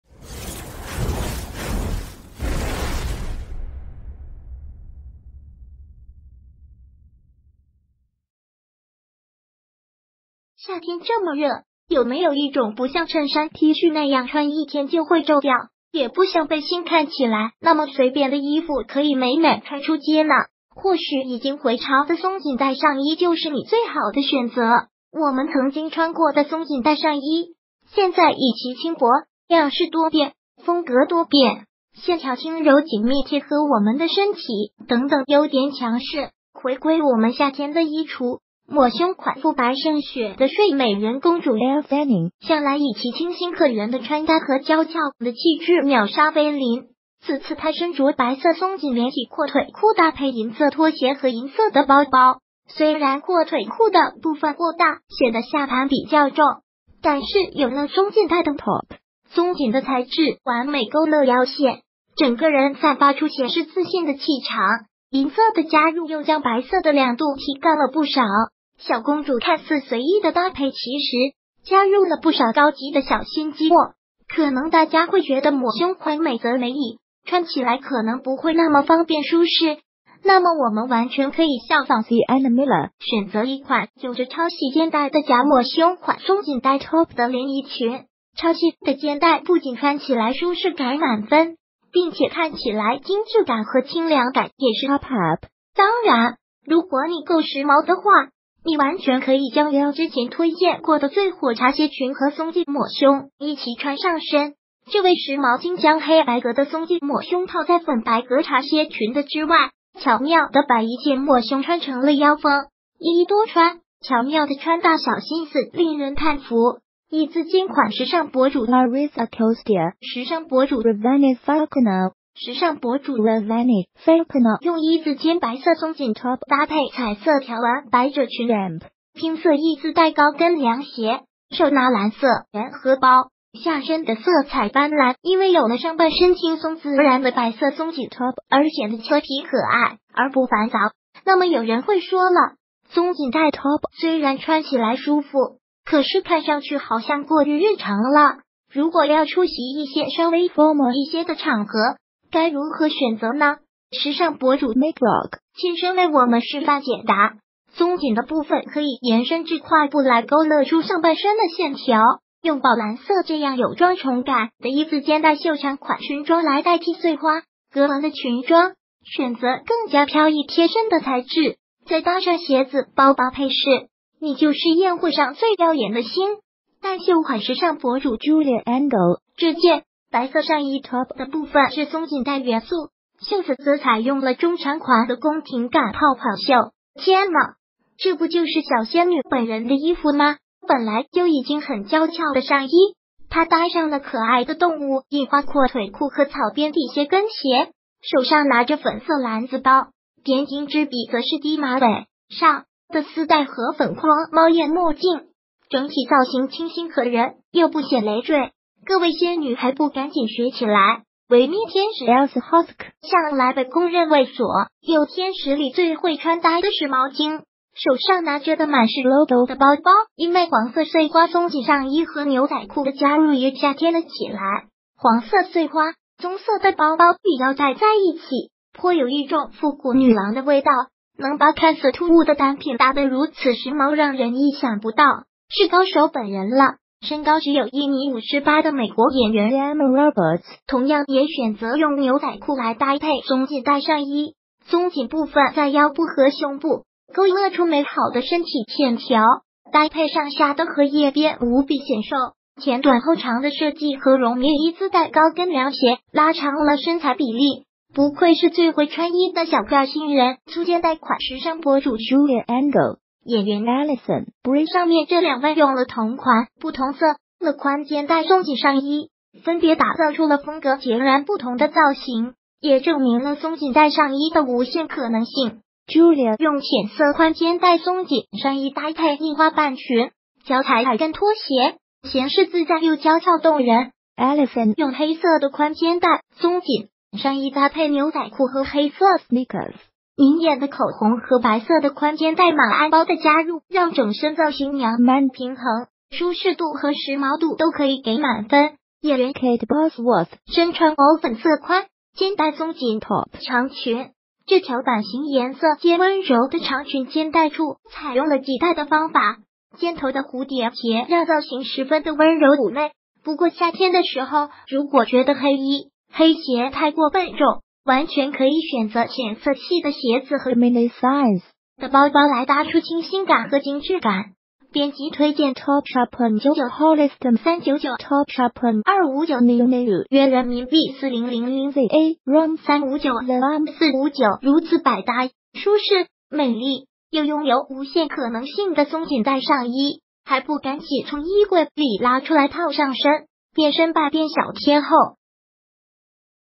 曾经穿过的土味松紧带上衣，又回潮了！
夏天这么热，有没有一种不像衬衫t恤那样穿一天就会皱掉，也不像背心看起来那么随便的衣服可以美美穿出街呢？或许已经回潮的松紧带上衣就是你最好的选择！我们曾经穿过的松紧带上衣，现在以其轻薄、样式多变、风格多变，线条轻柔紧密贴合我们的身体等等优点强势回归我们夏天的衣橱！抹胸款肤白胜雪的“睡美人”公主Elle Fanning向来以其清新可人的穿搭和娇俏的气质秒杀菲林，此次她身着白色松紧连体阔腿裤搭配银色拖鞋和银色的包包，虽然阔腿裤的部分过大显得下盘比较重，但是有了松紧带的top，松紧的材质完美勾勒腰线，整个人散发出闲适自信的气场。银色的加入又将白色的亮度提高了不少，小公主看似随意的搭配其实加入了不少高级的小心机哦！可能大家会觉得抹胸款美则美矣，穿起来可能不会那么方便舒适，那么我们完全可以效仿Sienna Miller选择一款有着超细肩带的“假抹胸”款松紧带top的连衣裙，超细的肩带不仅穿起来舒适感满分，并且看起来精致感和清凉感也是up up。当然，如果你够时髦的话，你完全可以将ELLE之前推荐过的最火“茶歇裙”和松紧抹胸一齐穿上身，这位时髦精将黑白格的松紧抹胸套在粉白格茶歇裙的之外，巧妙地把一件